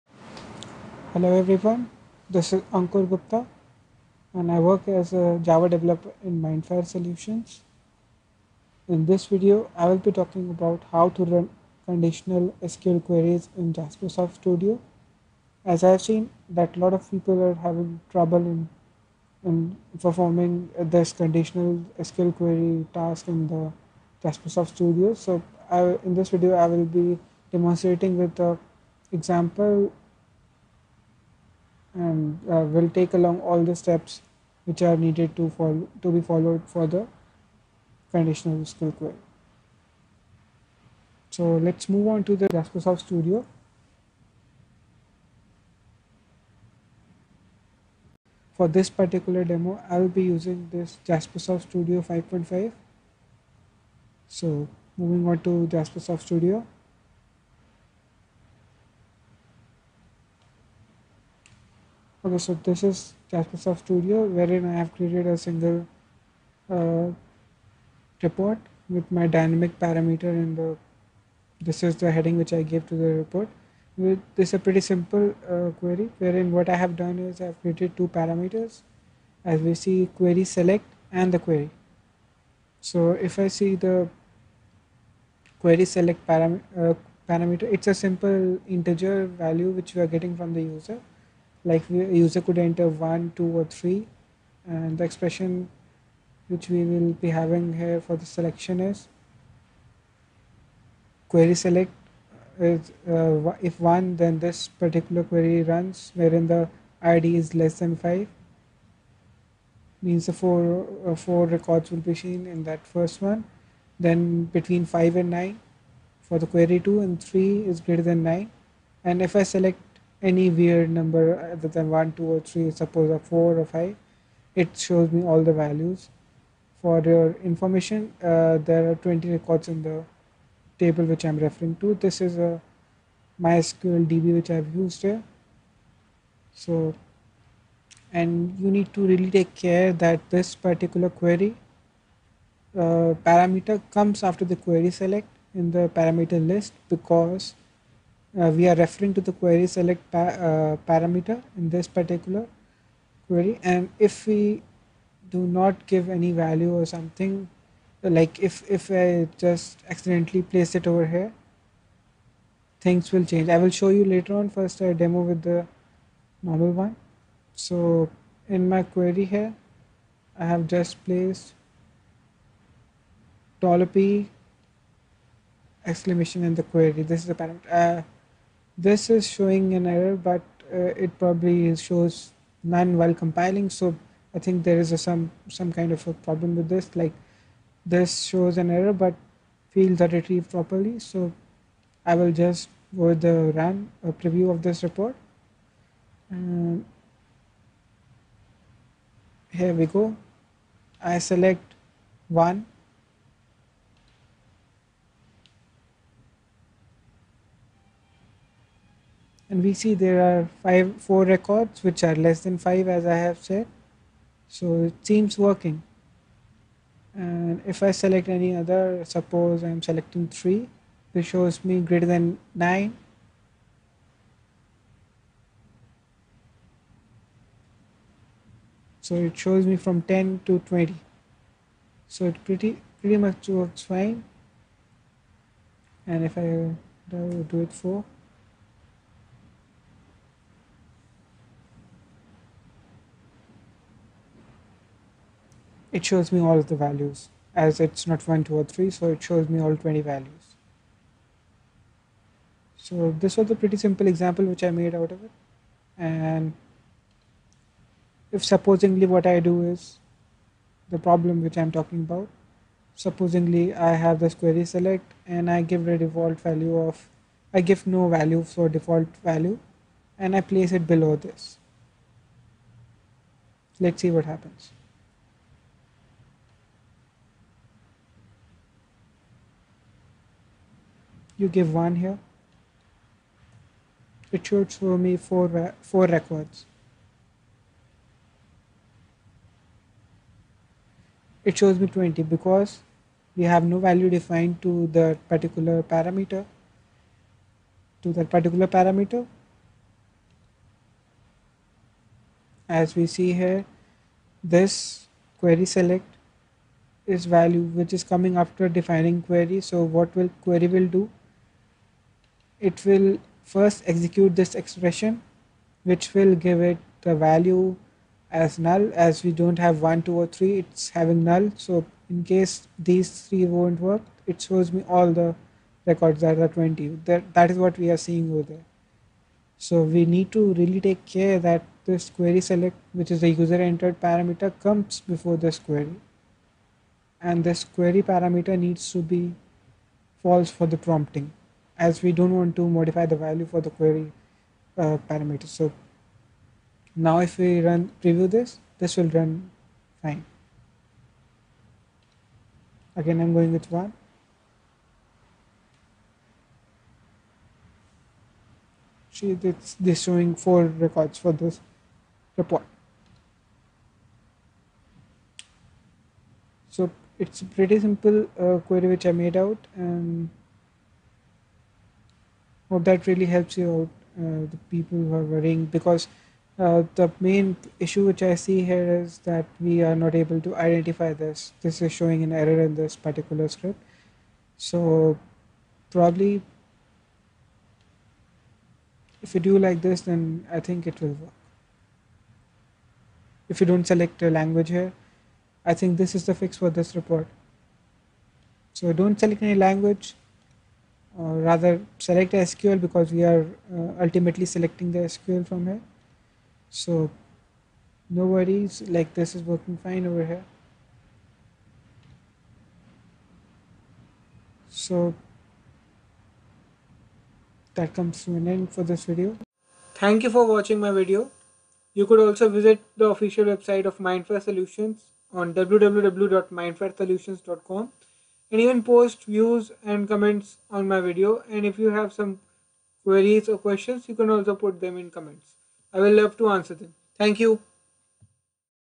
MFDynamicSQL Parameters in JasperSoft Studio
This video will allow the viewer to use Dynamic query using JasperSoft Studio. This video specifically deal with JasperSoft Studio.

Presenter: Ankur Gupta, Java Developer, Mindfire Solutions